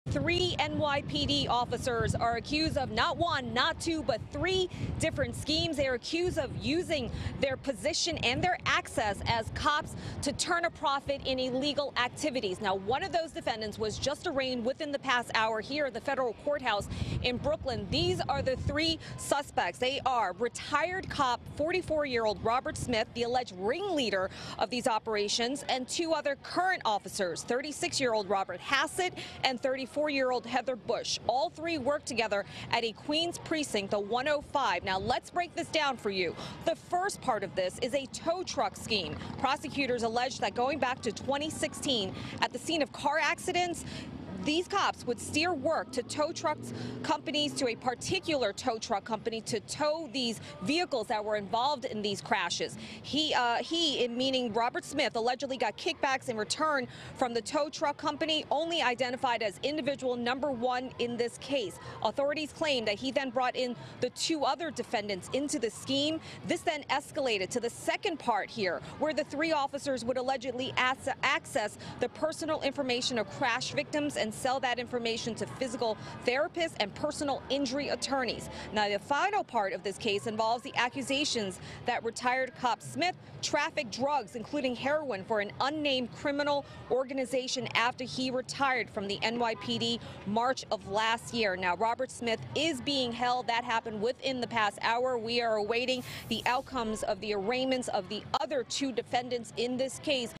NYPD officers arrested in alleged bribery scheme
Three current and former NYPD officers were arrested Tuesday for participating in bribery schemes, including selling victims’ information, according to federal prosecutors.